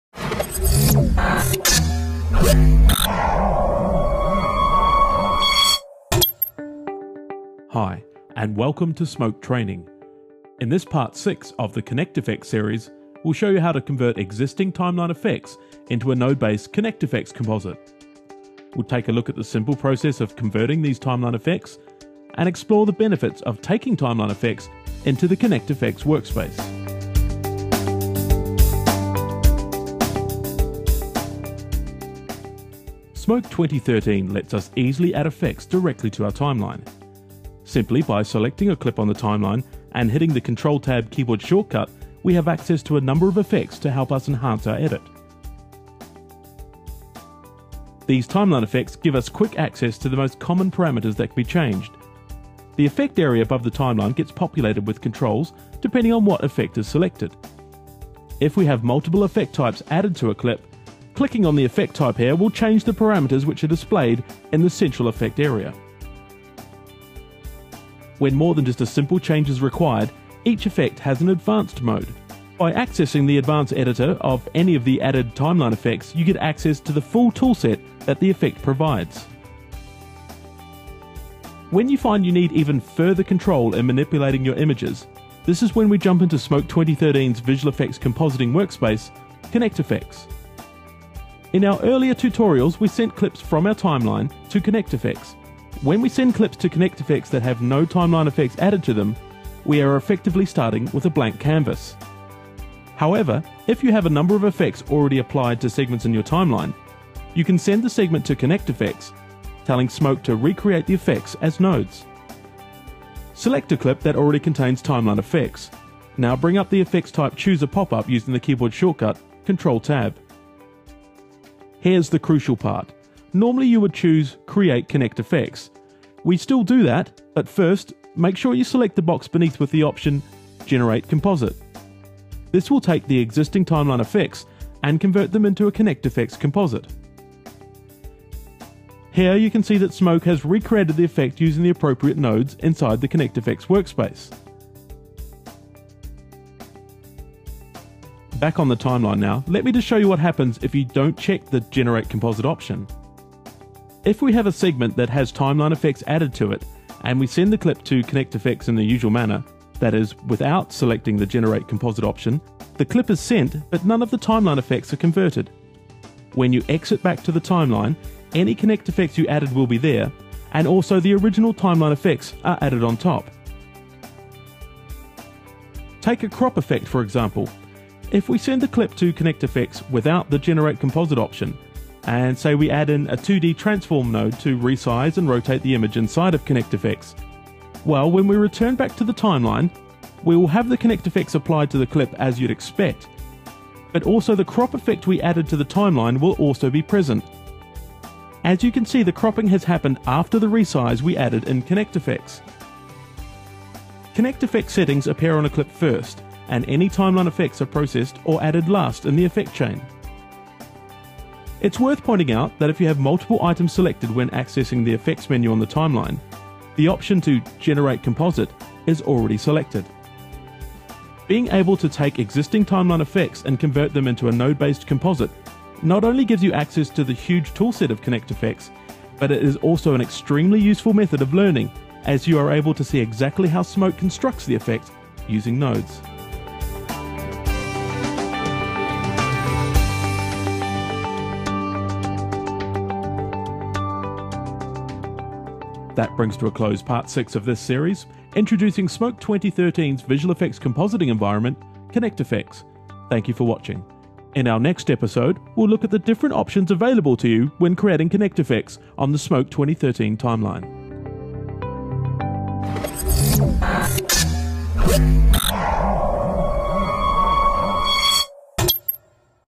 ST021 CFX PT6 - Converting Timeline FX
In this SMOKE training episode we'll take a look at existing timeline effects and converting them to SMOKE 2013′s visual effects compositing environment called ConnectFX.

In this part 6 of a multi-part series on ConnectFX, we will explore:

- The simple process of converting existing timeline effects
- And explore the benefits of taking your effect shots into ConnectFX

Follow @SMOKEtraining for all future tutorial updates